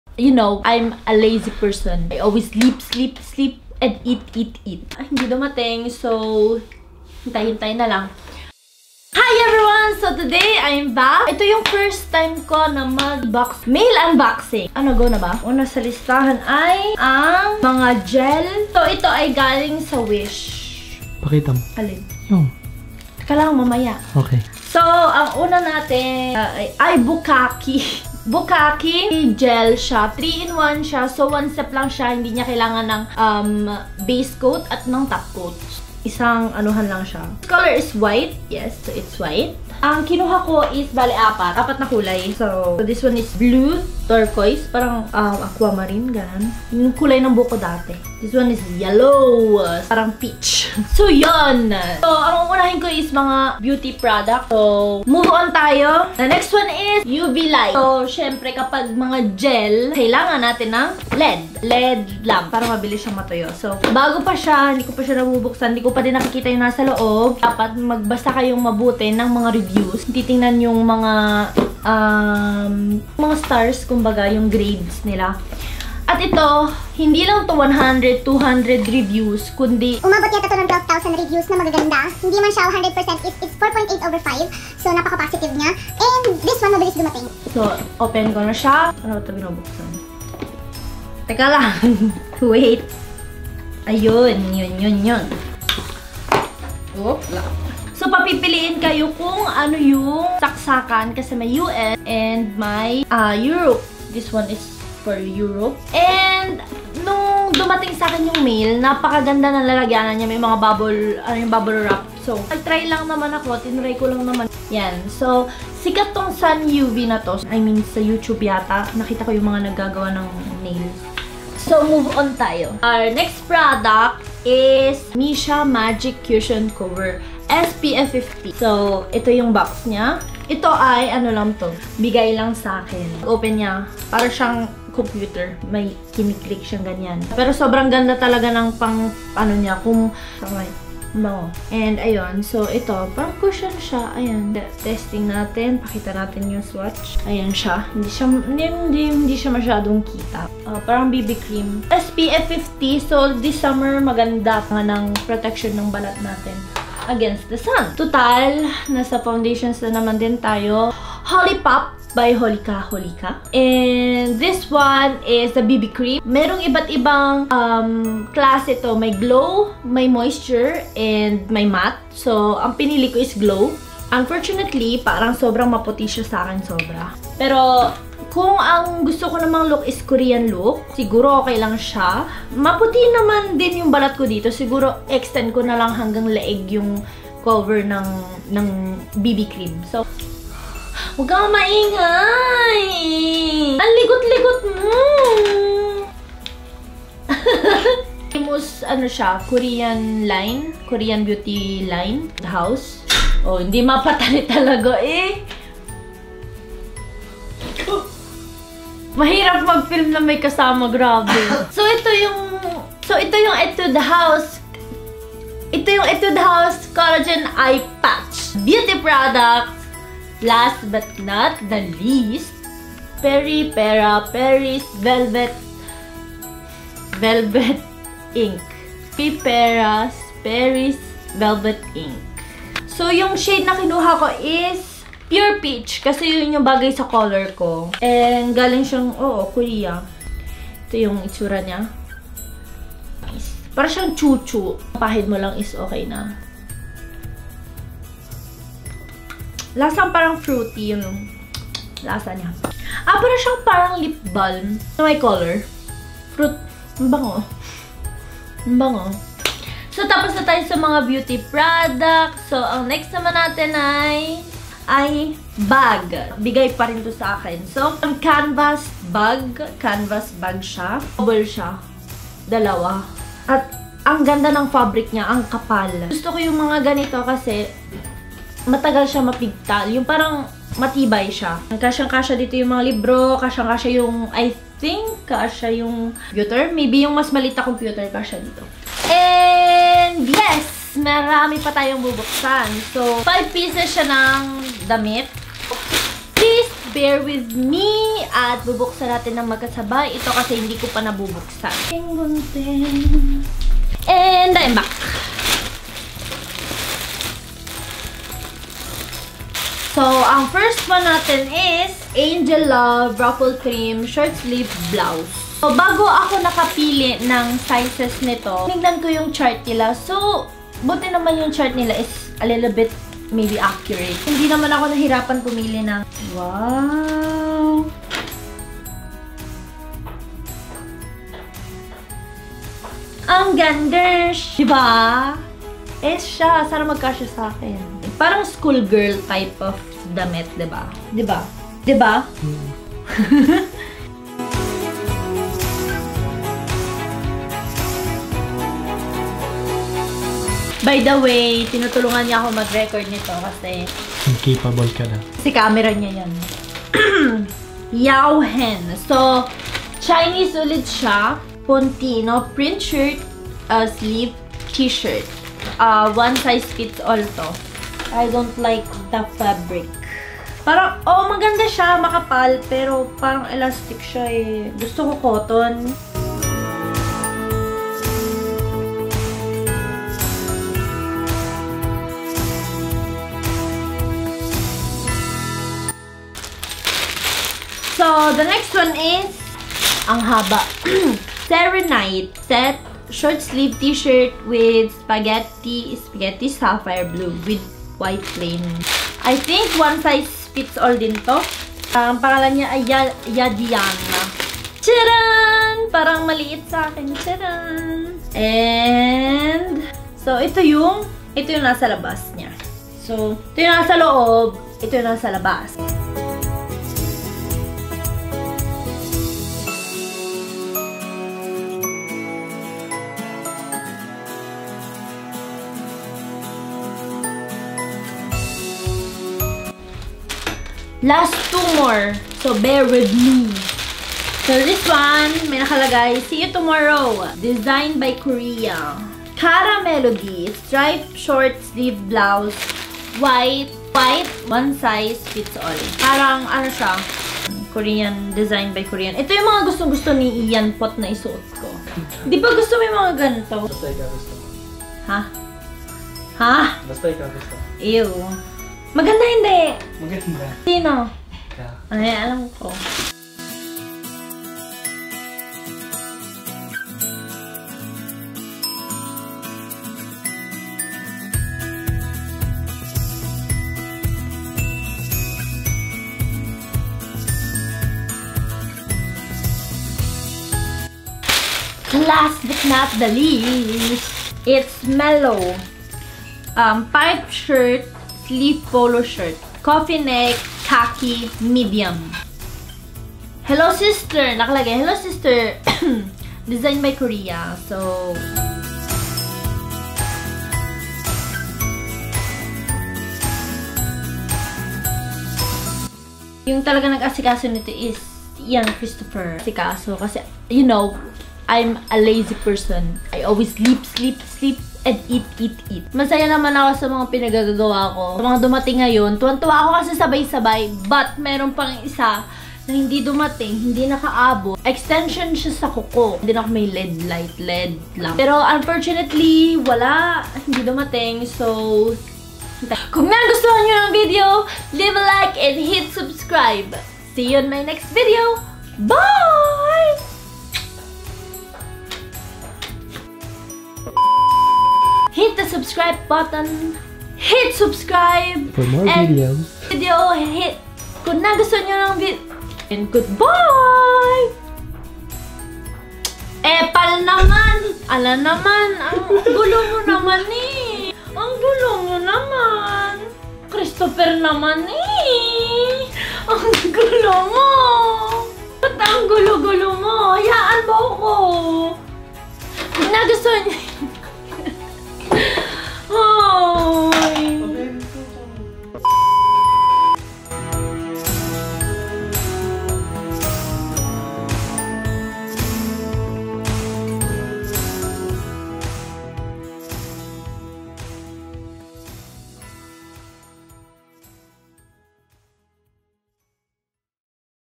Unboxing Time: Korean Products! + Try on
Hi guys! I edited this since last week but i had to postpone the uploading because it won't be consistent if i upload 2 videos in 1 week then no uploads for the next two, so here it is. ME UNBOXING SOME MAIL :)

and most of these products are GIFT-